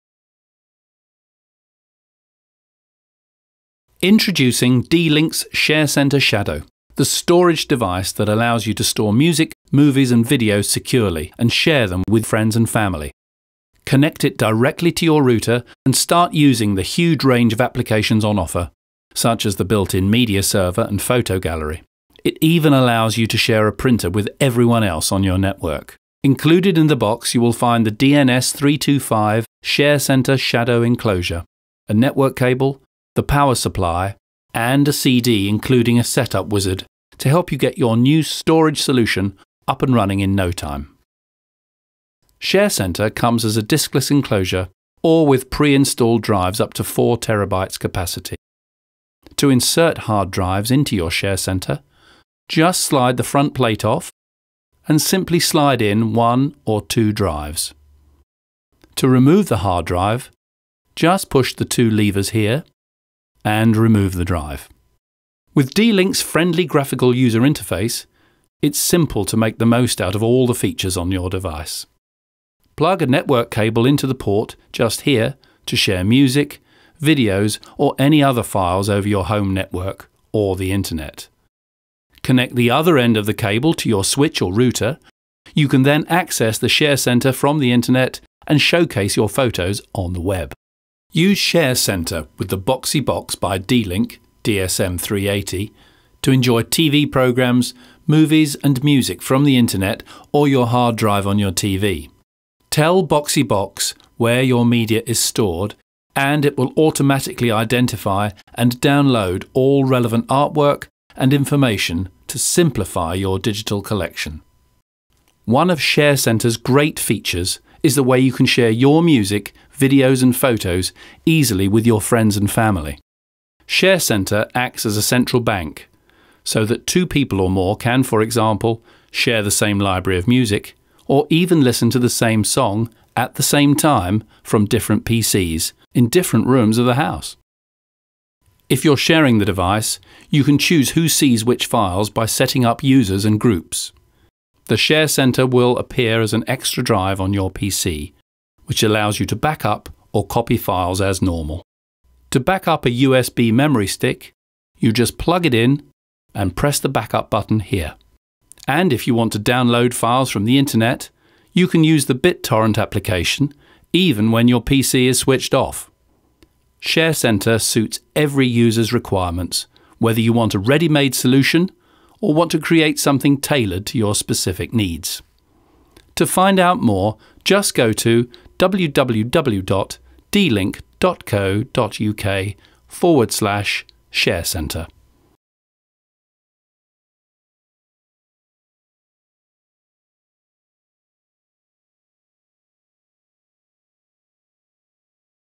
ShareCenter
The D-Link DNS-320 ShareCenterTM Pulse is a 2-bay Network Attached Storage (NAS) device with backup and media features that make it ideal for storing and sharing photos, music, movies and work files on a home network